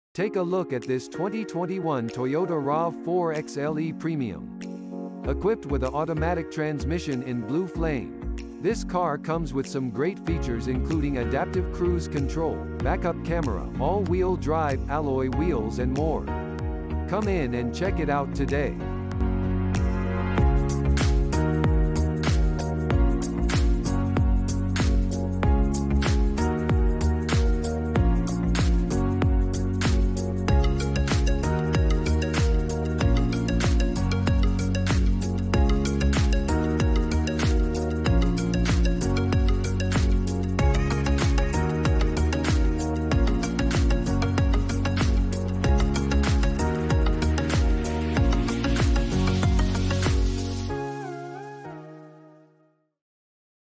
2021 Toyota Rav4 Xle Premium XLE PREM AWD SUV TT09864X
Take a look at this 2021 Toyota Rav4 Xle Premium! Equipped with a Automatic transmission in Blue Flame. This car comes with some great features including Adaptive Cruise Control, Backup Camera, All Wheel Drive, Alloy Wheels and more. Come in and check it out today!